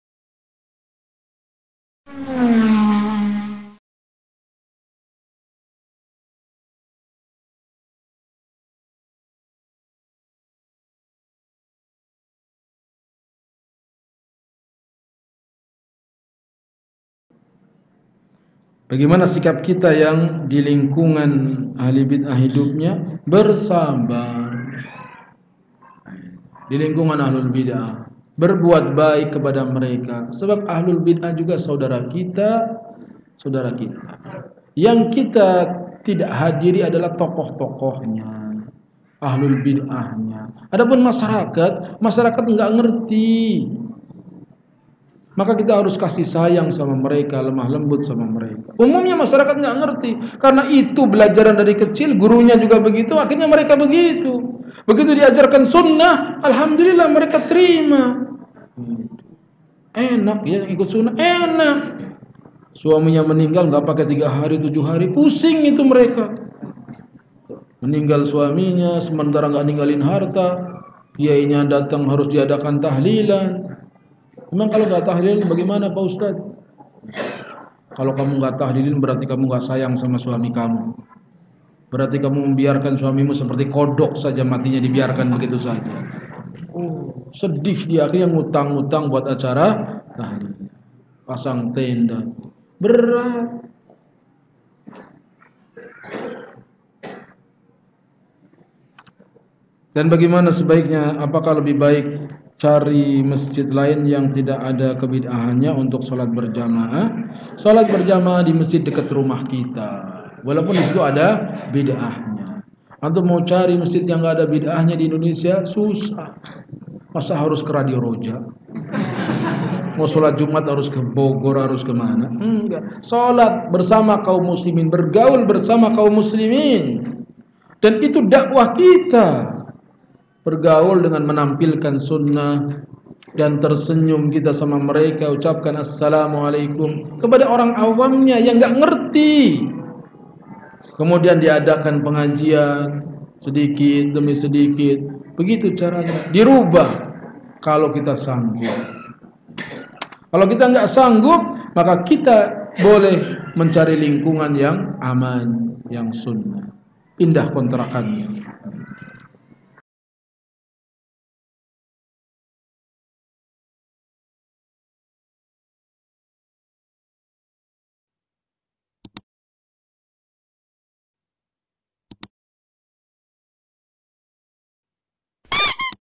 NO.266 TINGGAL DILINGKUNGAN AHLU BIDAH APA YANG HARUS KITA LAKUKAN ?
Apabila kita tinggul di lingkungan Ahlu Bid'ah, apa yang seharusnya kita lakukan ?